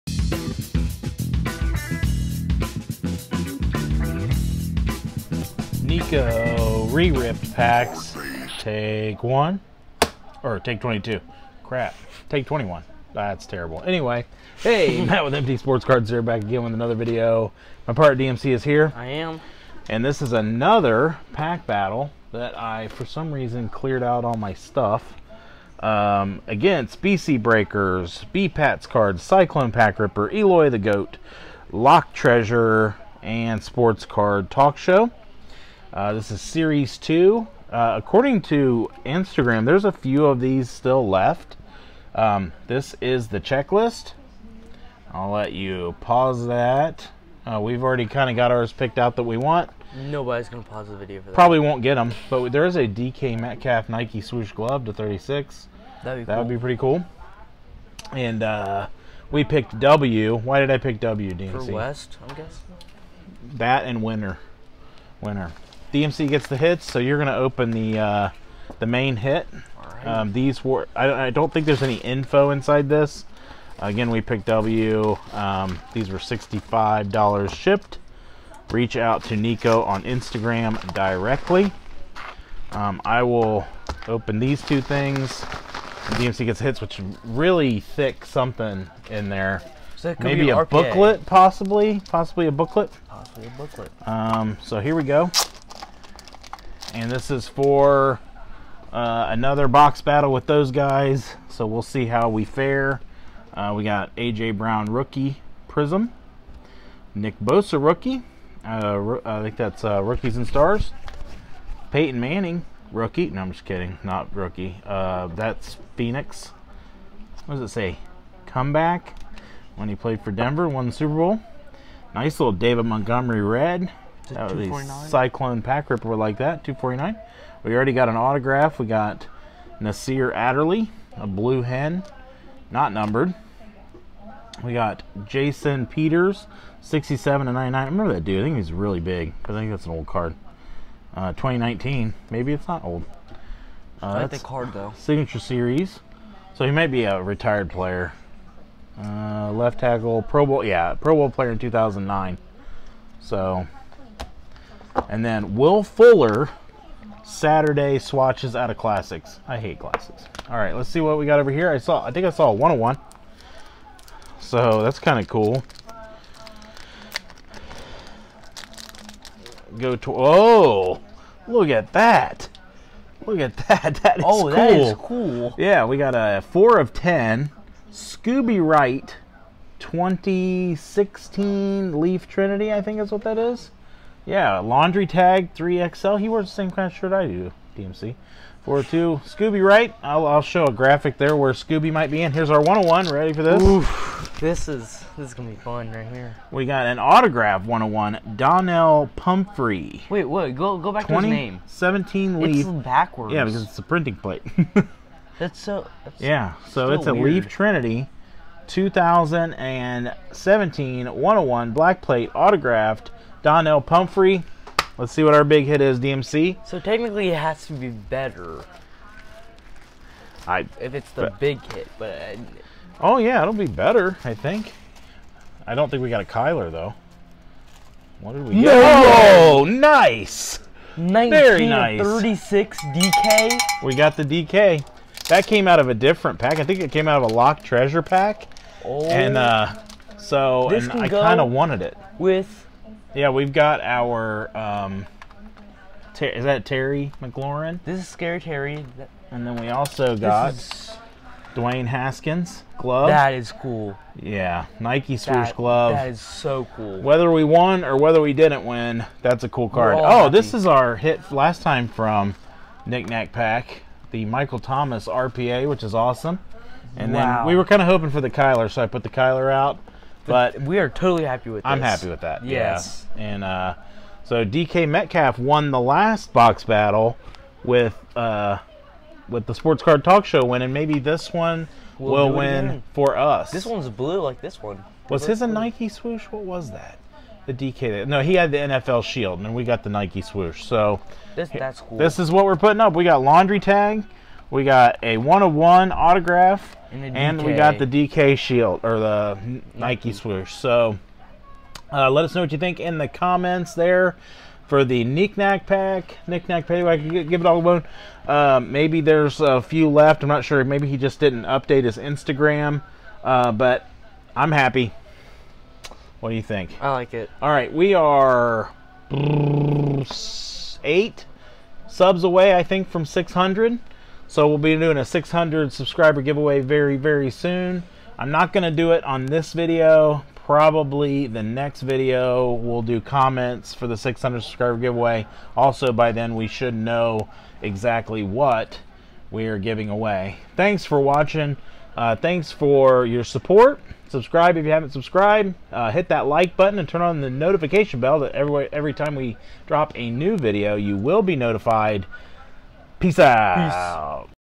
Nikko Rips Re-Ripped Version 2 - 🔥🔥🔥Top RC SP Sweet Card
Join DMC and I as we battle The following channels with a Nikko Rips football repack:
Bpats_cards
thesportscardtalkshow
Eloy the Goat
LKD Treasures
Cyclone Pack Ripper